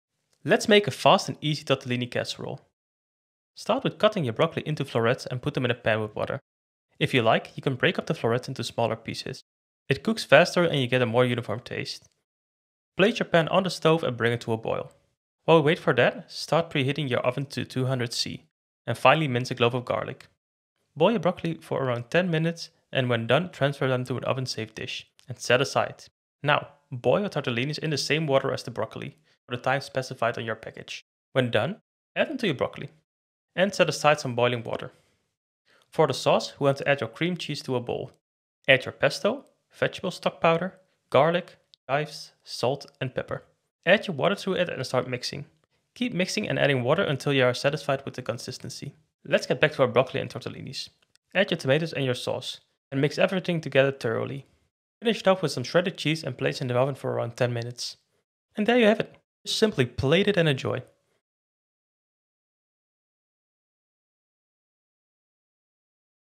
Tortellini Casserole on your plate in 30 minutes?
This tortellini casserole is on your plate in 30 minutes or less. It’s easy, fast and healthy! Perfect for weeknight’s!